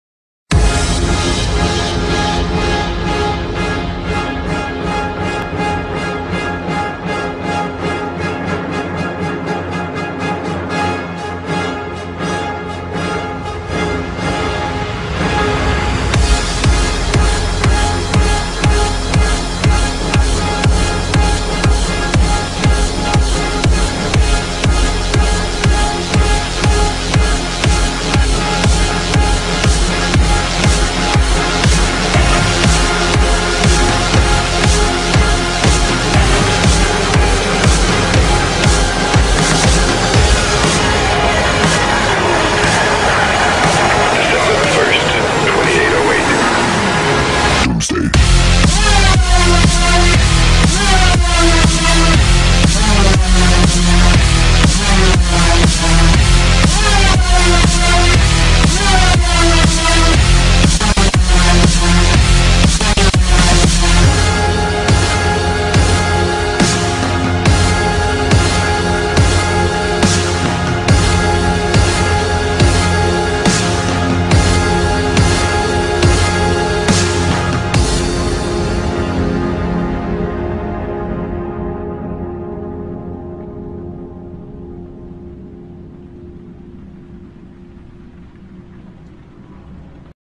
Half Ass Gaming: Kicking it up to 11!
Hey guys sorry for the lack of videos lately, we spent the last three days trying to recover 20 plus hours of recorded/edited footage. but the good news is we got it back and we all decided that from this point on we are going to kick things up to full auto!